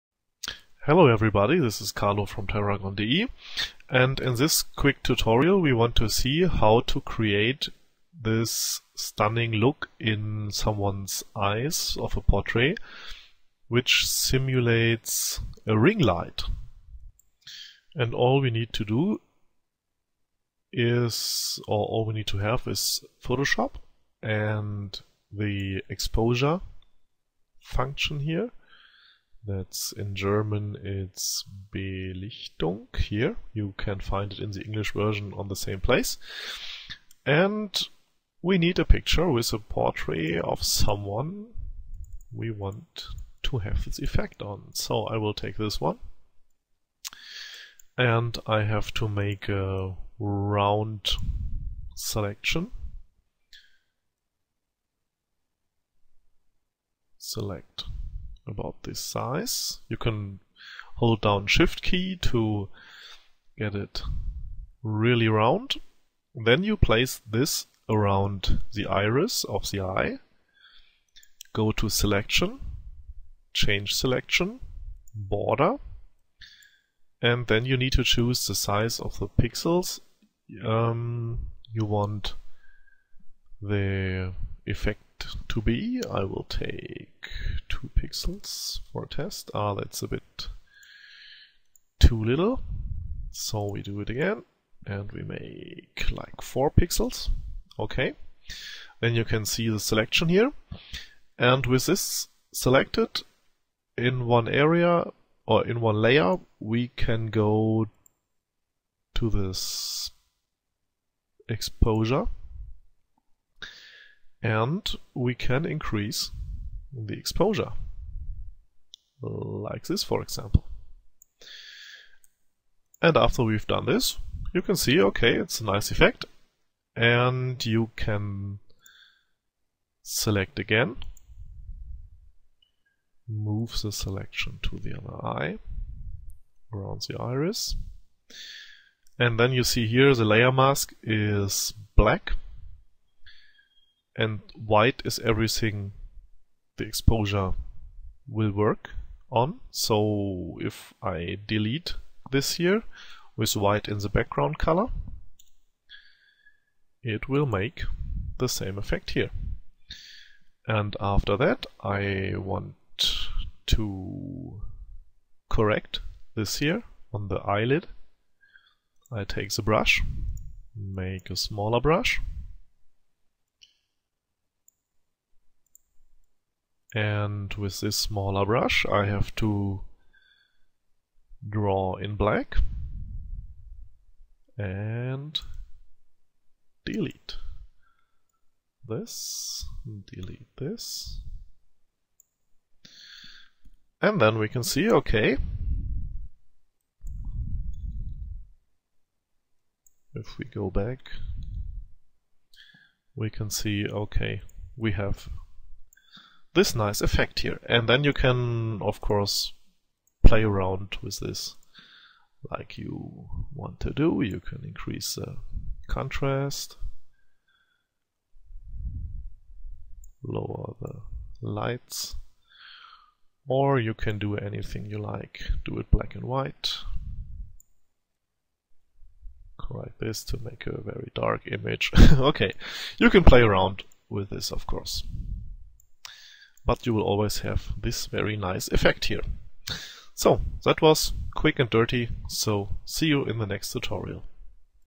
Photoshop Quick Tutorial: Ringlight / Ringblitz effect in the Eye
Welcome to my next tutorial :-)

In this tutorial we will see how to:

1. Use a regular portait photo
2. select the circle around the iris
3. change the selection to be a round "tube"
4. add the exposure effect to the selection in a new effect layer with a layermask
5. delete the areas around the eye where we do not want to have the effect
6. move the duplicated object to the left to create the 3D effect that we want
7. done :-)

Required and used Software:
- Adobe Photoshop CS5
- a portrait

I hope that you will enjoy this basic tutorial!